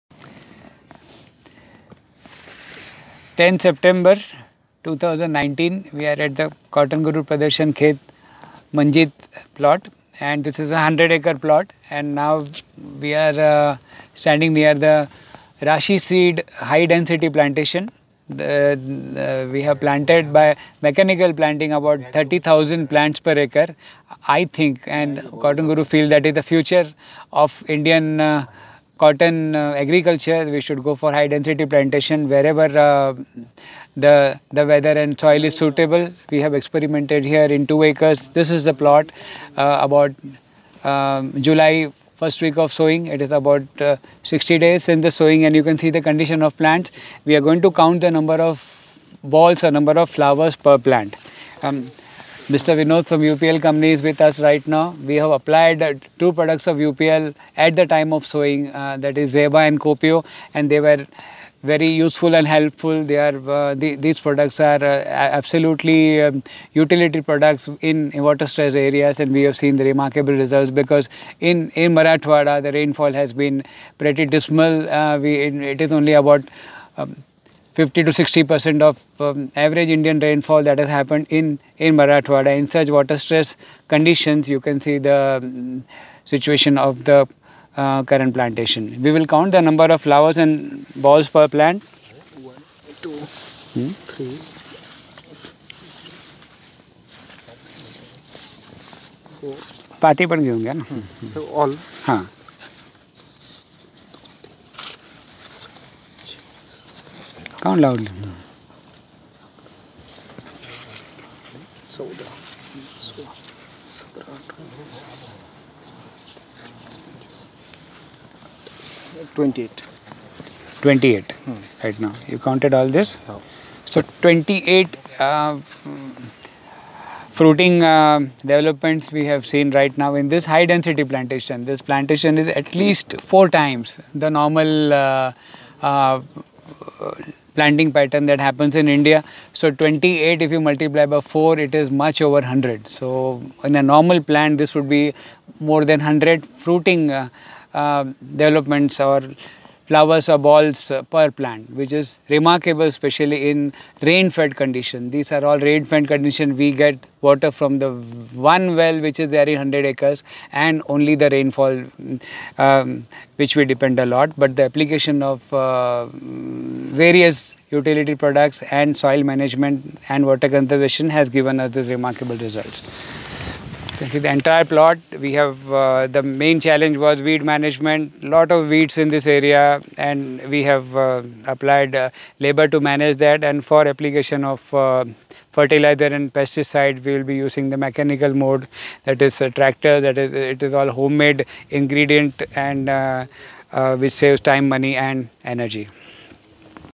COTTONGURU® Midas Touch farm Gevrai Tanda, Paithan road, Aurangabad
Area=100acres, Status: Dry, Rainfall=Low,     Land Owner= Manjeet Group, Seed = RASI Growth Rulators (Zeba,Copio)=UPL,
USP= One of the 1st High density plantation with mechanical planting in Marrathawada
Special Mention: Good support of UPL growth regulators